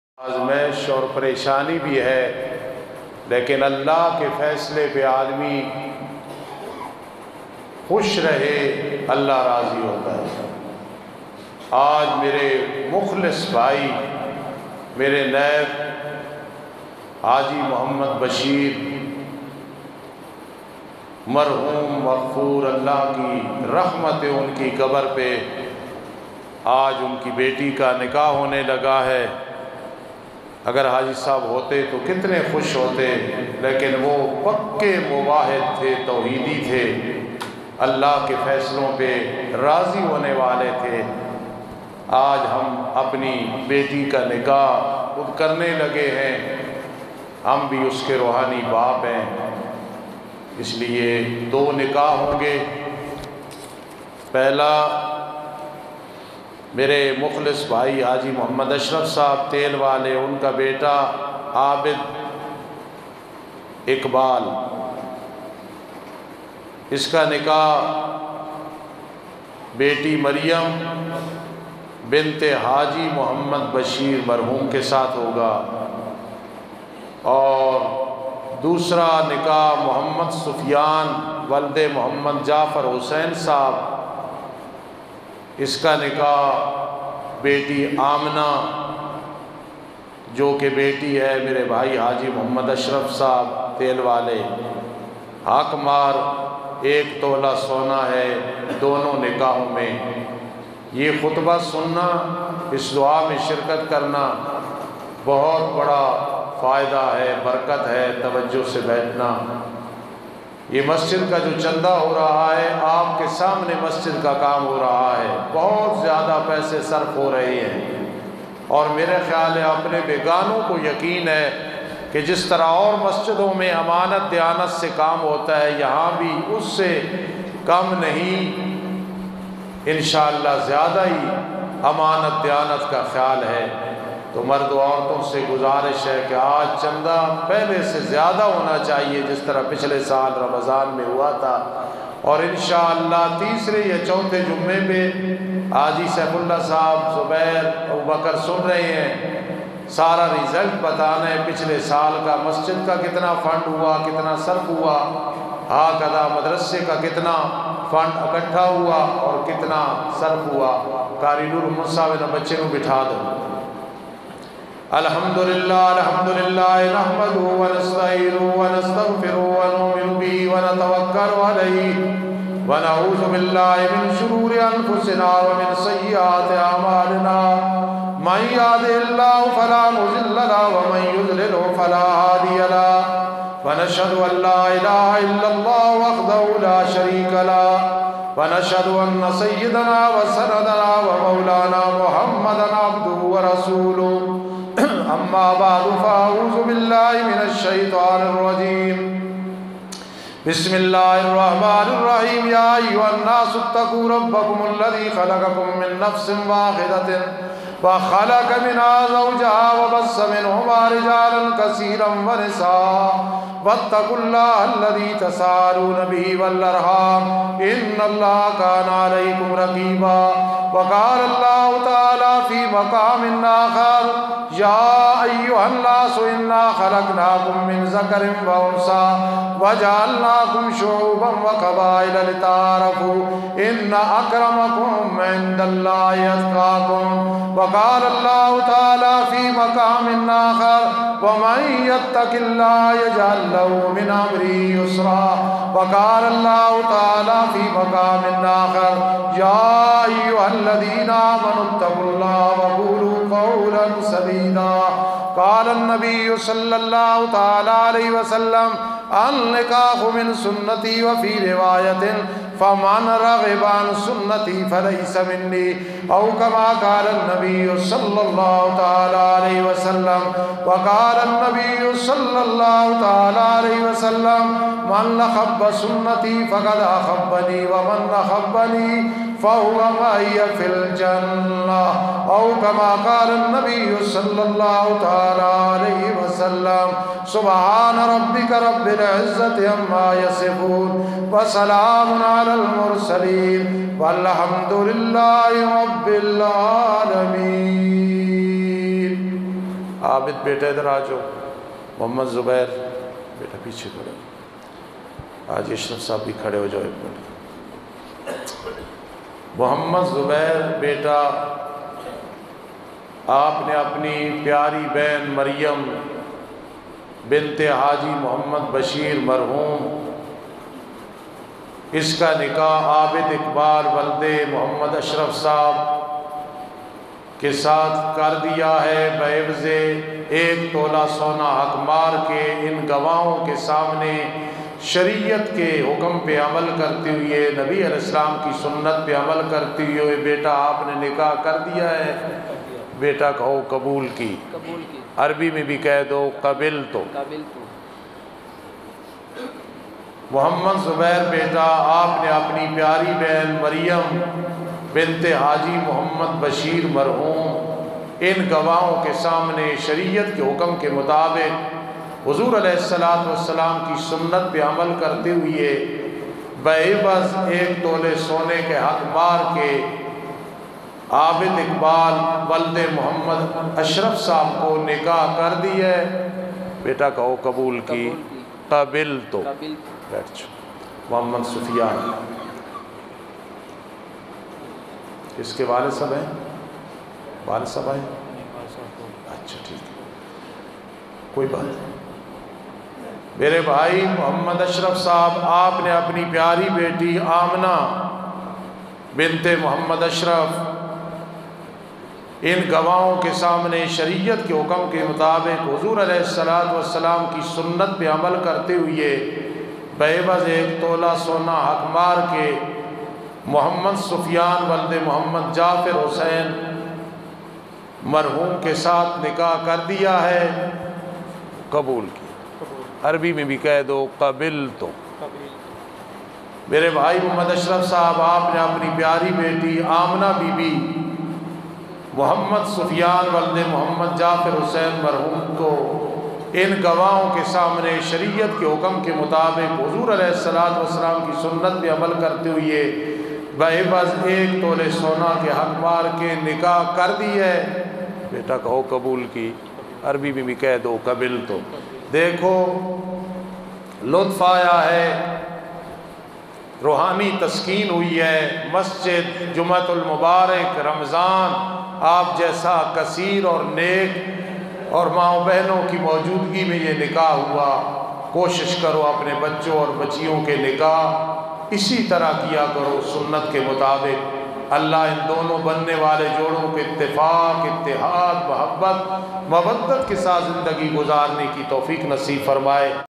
Gulshan e farooqi me Ramzan k pehly juma pe aham sunnat ko zinda kia gya,6th ramzan2022
ادارہ تسجیلات الفاروقیہ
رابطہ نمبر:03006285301